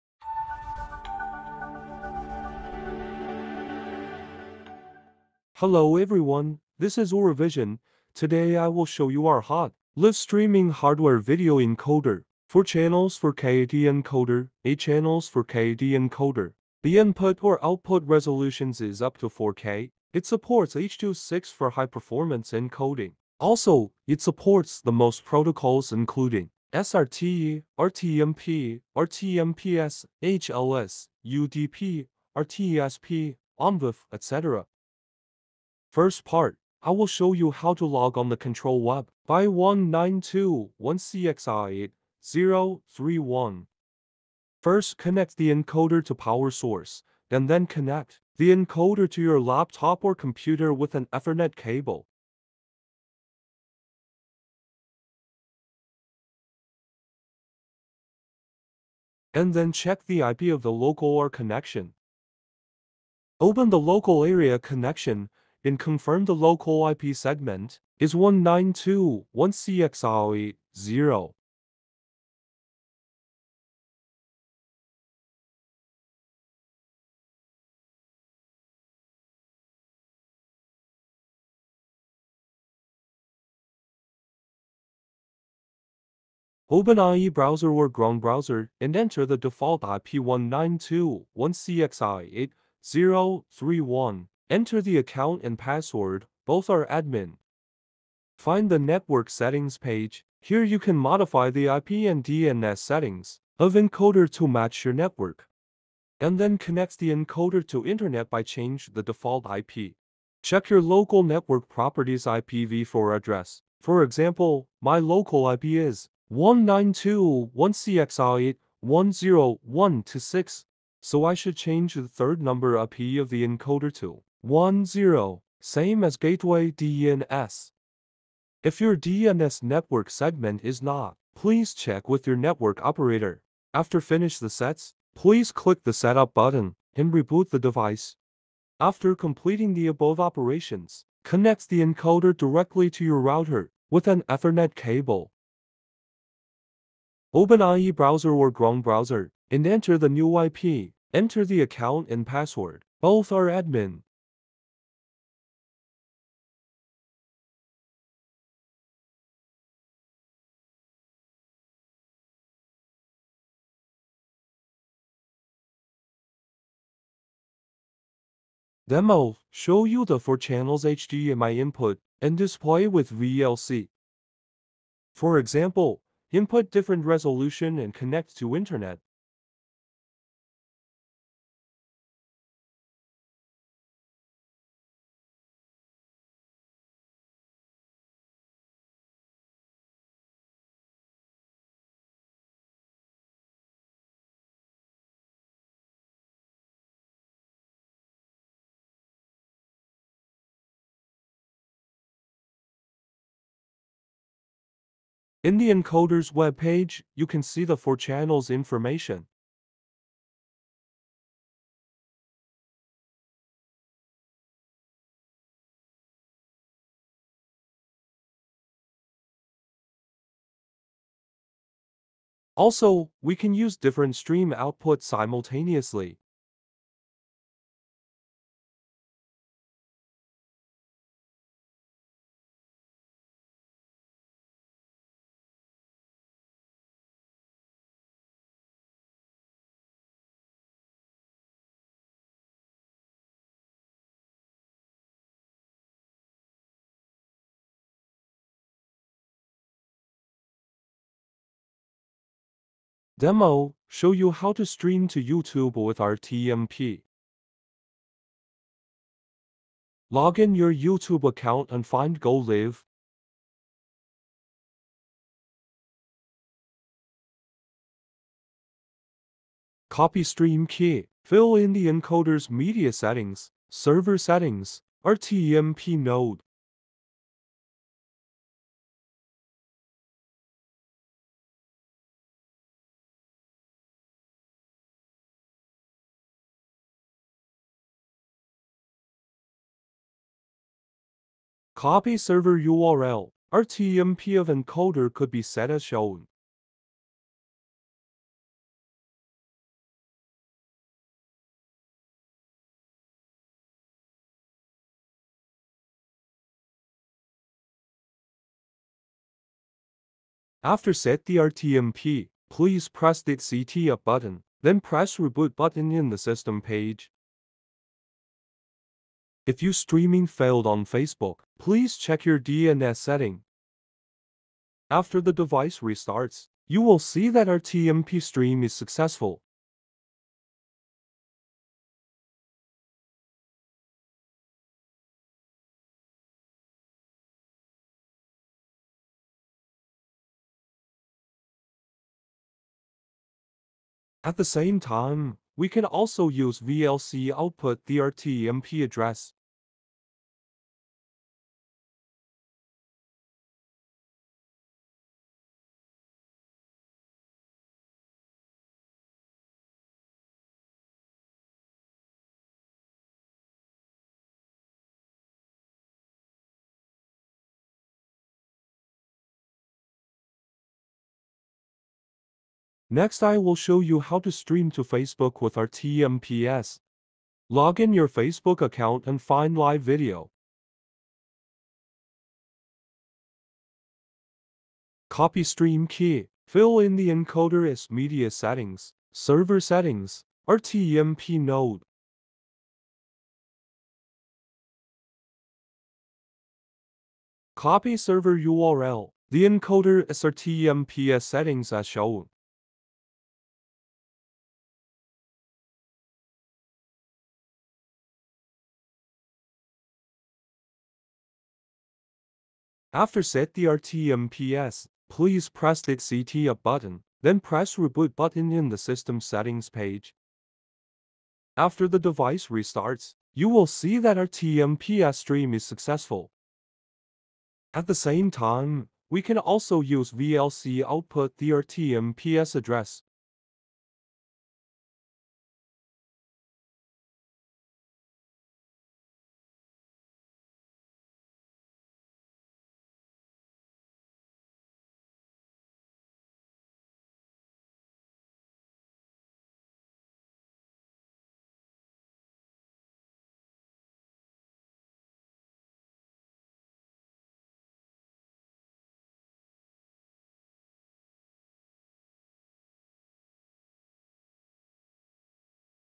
ORIVISION 4/8 Channels 4K@30Hz H.264 HDMI Video Encoder
ORIVISION’s UHD 4K video encoder which is designed to meet the needs of all 4K video transmission applications.

The input/output resolutions is up to 4/8 channels 4Kp30.
It supports H.264 high-performance encoding.
Also, it supports the most protocols including SRT/RTMP/RTMPS/HLS/UDP/RTSP/Onvif, etc.
You can send you encoded video to live streaming platform like youtube, facebook, wowza, etc.
Apply to TV programming, lecture capture, online courses, remote learning, course recording, broadcast meetings, Web training, remote pathology, telemedicine, conferences, concerts, sports.